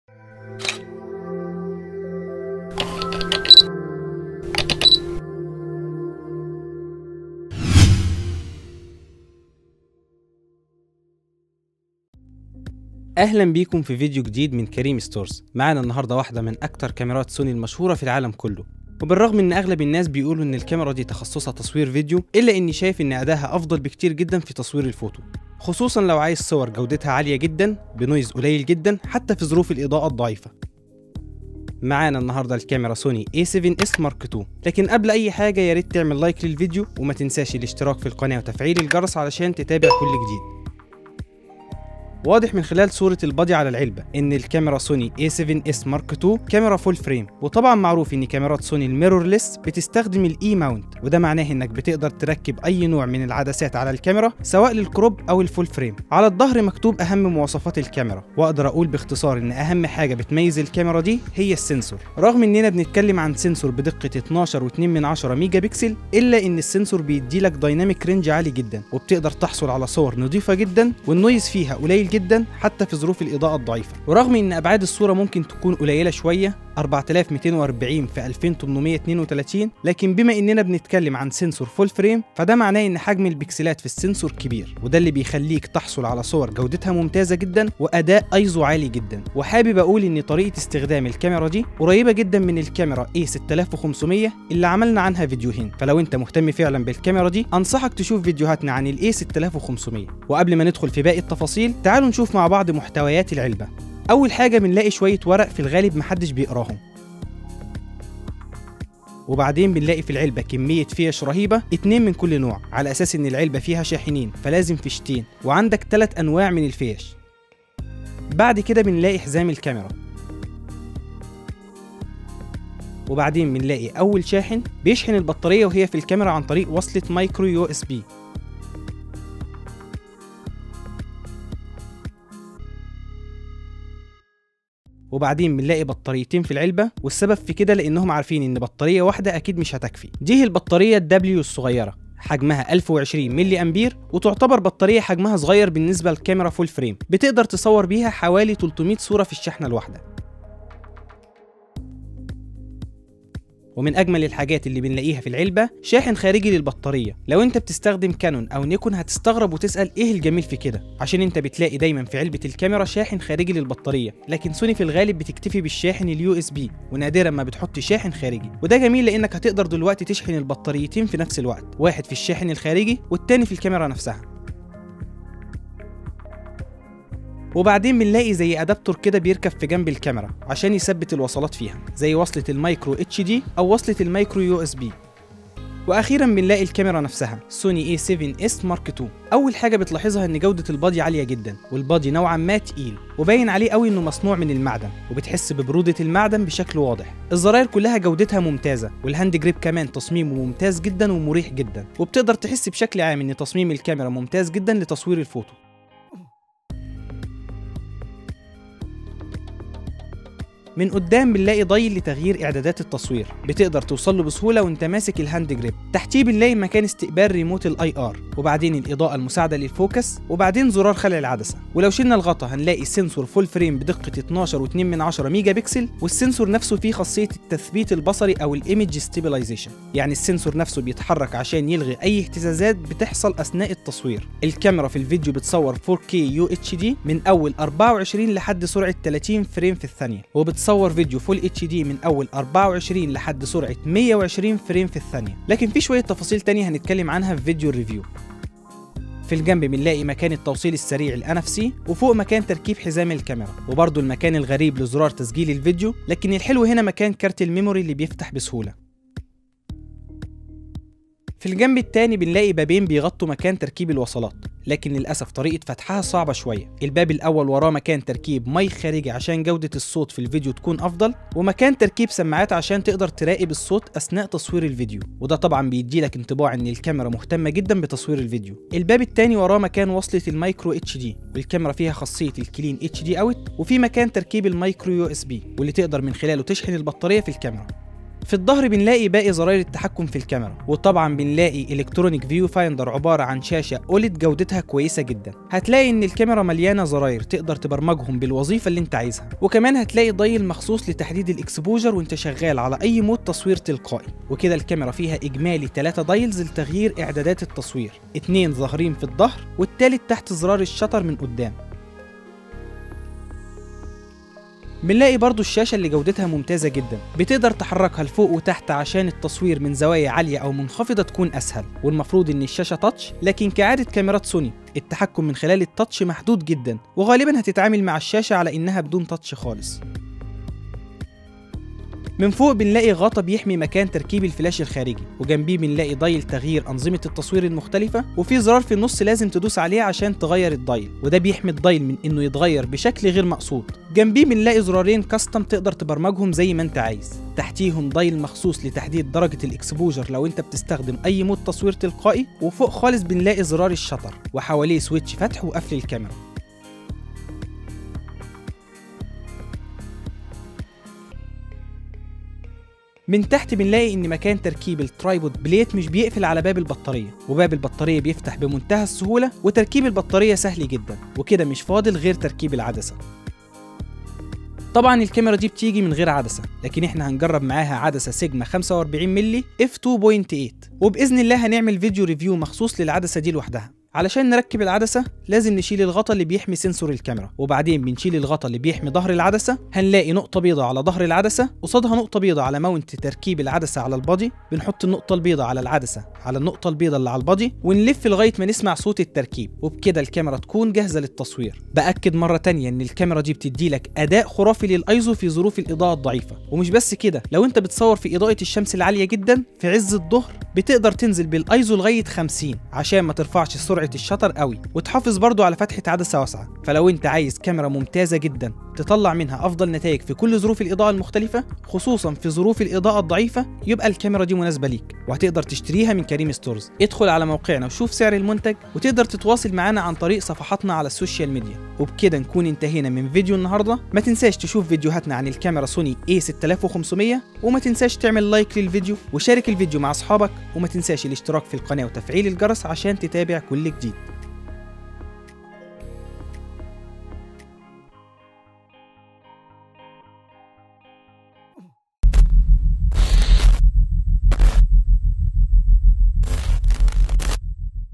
أفضل كاميرا في ظروف الإضاءة الضعيفة Sony Alpha a7S II Unboxing & Specs
أفضل كاميرا في ظروف الإضاءة الضعيفة
Sony Alpha a7S II Unboxing & Specs

معانا النهاردة واحدة من أكتر كاميرات سوني المشهورة في العالم كله. وبالرَّغم إن أغلب الناس بيقولوا إن الكاميرا ديه تخصصها تصوير فيديو. إلا إنِّي شايف إن أداءها أفضل بكتير جداً في تصوير الفوتو، خصوصاً لو عايز صور جودتها عالية جداً، بنويز قليل جداً، حتى في ظروف الإضاءة الضعيفة. واقدر أقول باختصار إن أهم حاجة بتميِّز الكاميرا دي هي السينسور. رغم إننا بنتكلِّم عن سينسور بدقة 12.2 ميجا بكسل، إلَّا إن السينسور بيديلك دايناميك رينج عالي جداً، وبتقدر تحصل على صور نضيفة جداً، النويز فيها قليل جداً، حتى في ظروف الإضاءة الضعيفة. ورغم إن أبعاد الصور مُمكن تكون قليلة شوية: 4240 * 2832، لكن بما إننا بنتكلم عن سينسور فل فريم، فده معناه إن حجم البيكسلات في السينسور كبير، وده اللي بيخليك تحصل على صور جودتها مُمتازة جداً، وأداء أيزو عالي جداً، أول حاجة بتلاحظها إن جودة البودي عالية جداً، والبودي نوعاً ما تقيل، وباين عليه أوي انه مصنوع من المعدن، وبتحس ببرودة المعدن بشكل واضح، الزراير كلها جودتها مُمتازة، والهاند جريب كمان تصميمه مُمتاز ومُريح جداً، وبتقدر تحسّ بشكل عام إن تصميم الكاميرا مُمتاز جداً لتصوير الفوتو، ولو شيلنا الغطا، هنلاقي سينسور فل فريم بدقة 12.2 ميجا بكسل، والسينسور نفسه فيه خاصية التثبيت البصري، أو الإميدج ستيبلايزيشن، يعني السينسور نفسه بيتحرك عشان يلغي أي اهتزازات بتحصل أثناء التصوير، الكاميرا في الفيديو بتصوَّر 4K UHD، من أول 24 لحد سرعة 30 فريم في الثانية، وبتصوَّر فيديو FHD من أول 24 لحد سرعة 120 فريم في الثانية، بأكِّد مرة تانية إن الكاميرا ديه بتديلك أداء خرافي للأيزو في ظروف الإضاءة الضعيفة، ومش بس كده، لو انت بتصور في إضاءة الشمس العالية جداً، في عزّ الظهر، بتقدر تنزل بالآيزو لغاية 50، عشان مترفعش سرعة الشاتر أوي، وتحافظ برضه على فتحة عدسة واسعة، فلو انت عايز كاميرا مُمتازة جداً، تطلَّع منها أفضل نتايج في كل ظروف الإضاءة المُختلفة.

Heliopolis: 29 hegaz street beside farahat
Dokki: 73 Iran Street from Mohi Eldin Abo Elaz